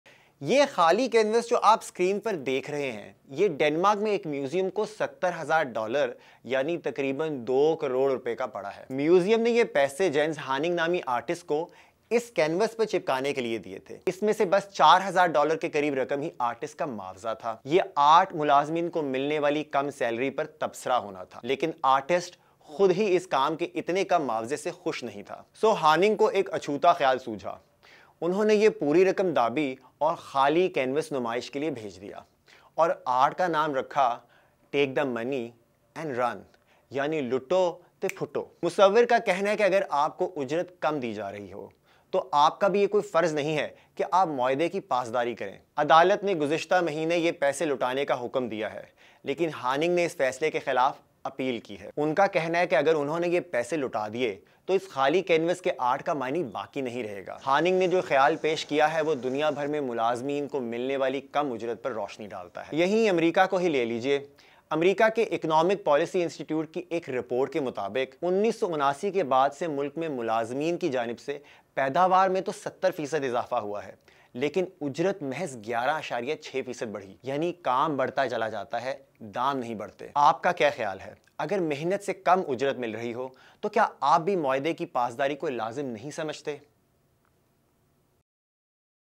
Danish artist sends blank canvas as art; terms it, "Take the money and run"
ڈینش آرٹسٹ نے ستر ہزار ڈالر کے بدلے خالی کینوس نمائش میں بھیج دیا۔ عدالت کا کہنا ہے کہ پیسے واپس کیے جائیں جب کہ آرٹسٹ کا موقف ہے کہ پیسے واپس کیے تو ان کے آرٹ، ’’پیسے دابو، اور بھاگو‘‘ کا مطلب نہیں رہے گا۔ ملک عمید کی رپورٹ